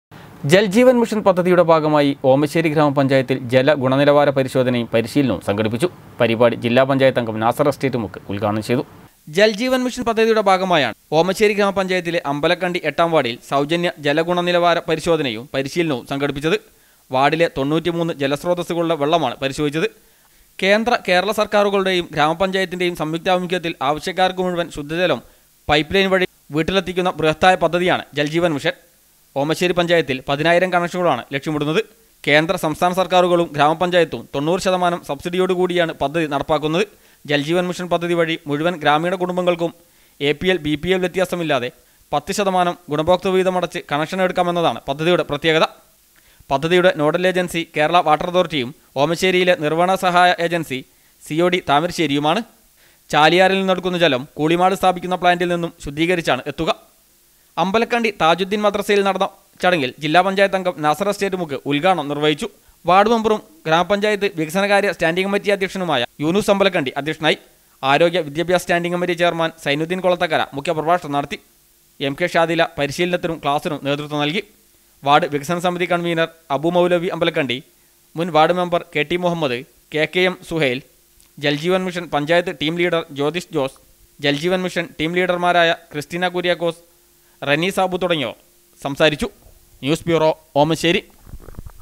ഓമശ്ശേരി ഗ്രാമപഞ്ചായത്തിൽ ജലഗുണനിലവാര പരിശോധനയും പരിശീലനവും സംഘടിപ്പിച്ചു.
ജൽ ജീവൻ മിഷൻ പദ്ധതിയുടെ ഭാഗമായി ഓമശ്ശേരി ഗ്രാമപഞ്ചായത്തിൽ ജലഗുണനിലവാര പരിശോധനയും പരിശീലനവും സംഘടിപ്പിച്ചു. പരിപാടി ജില്ല പഞ്ചായത്തംഗം നാസർ എസ്റ്റേറ്റ് മുക്ക് ഉദ്ഘാടനം ചെയ്തു